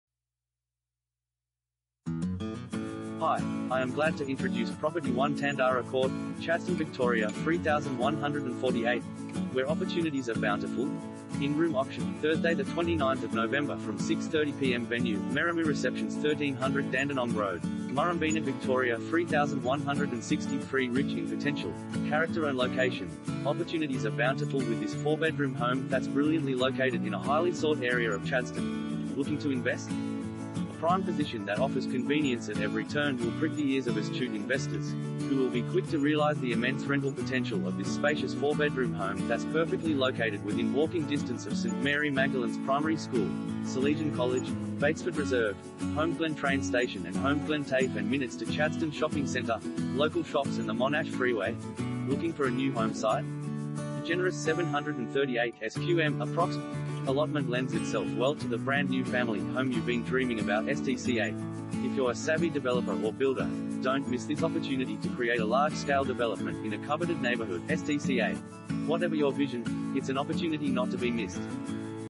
1 Tandara Court, Chadstone VIC 3148 | AuPropertyReport.Com
1 Tandara Court, Chadstone VIC 3148
Property Type: house
Bedrooms: 3
Bathrooms: 3
Car Space: 2
Where Opportunities Are Bountiful